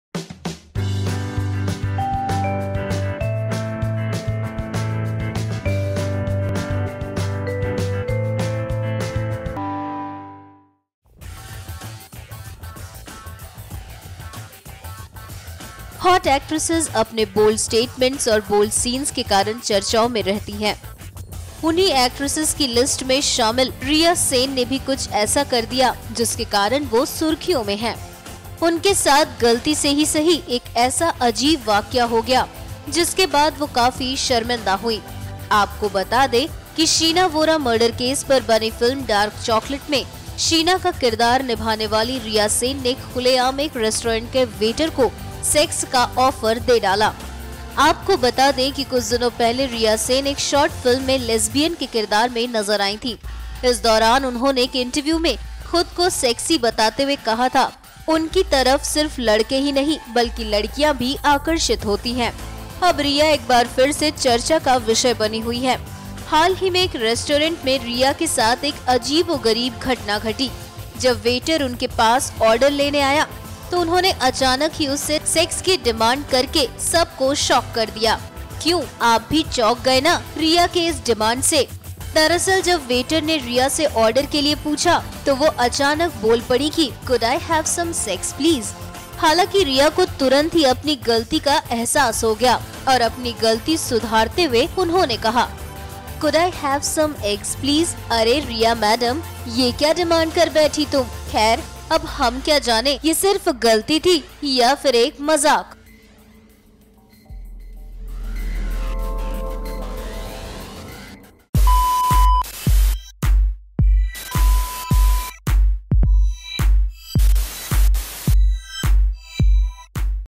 OMG! Hotel में Riya Sen ने की ऐसी चीज़ की Demand , Waiter सुन कर हो गया Shocked
As per a report Riya Sen who worked in Films like Style, Shaadi No 1, Silsilaay and others had gone out to have food in a popular restaurant in Andheri, Mumbai. The restaurant was known for saucily named cocktail, the popular Sex on the Beach, as well as a similarly named dish made from eggs, called Eggs on the Beach (it’s another matter that this one does not make any kind of sense). Well, what happened was Riya was in no mood for cocktails but for eggs, but the name of the . So when the waiter came to take down her order, Riya, who, at that time was dining with her friend, simply asked, “Could I have some SEX please?”As per the article the waiter was completely nonplussed by her question, she realised her gaffe and rectified her ‘order’, “Could I have some eggs please?”